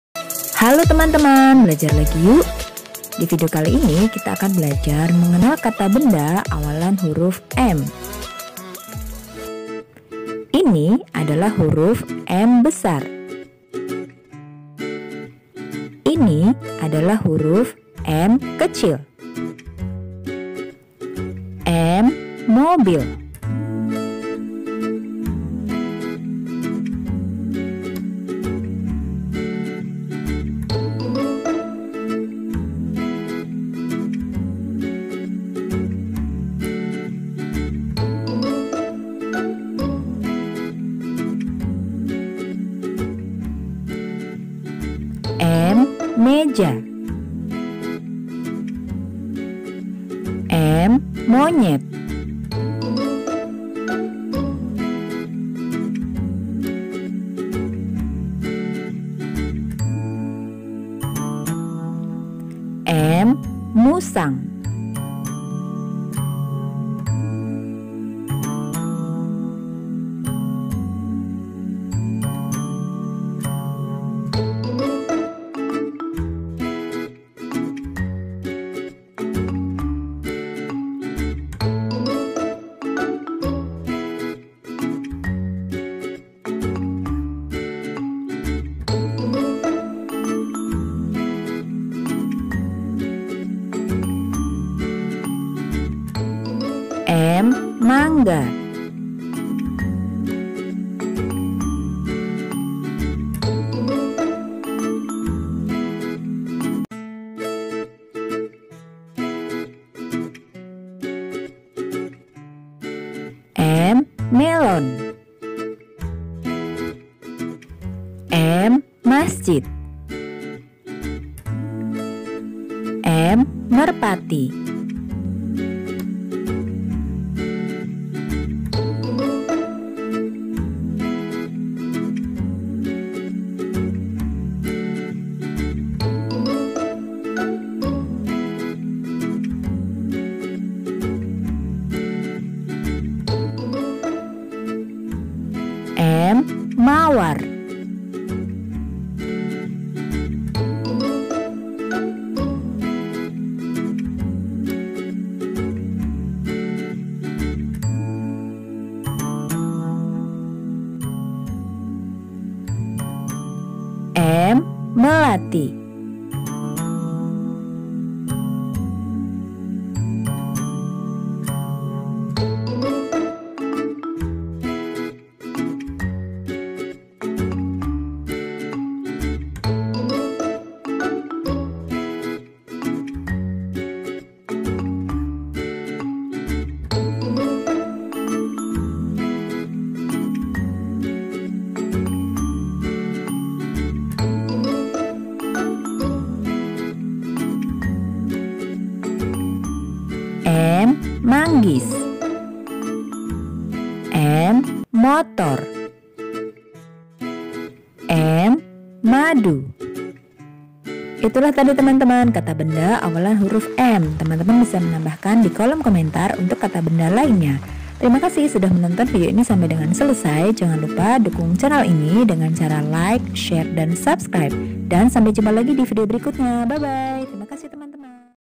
Mengenal Kata Benda Huruf M
Mengenal kata benda huruf M dapat membantu belajar menambah  kosa kata baru. Kata benda merupakan pengetahuan dasar dalam pelajaran bahasa. Melalui video ini kita dapat belajar mengenal huruf M besar dan huruf m kecil,  mampu mengenal dan menyebutkan nama atau kata benda awalan huruf m.

Terima kasih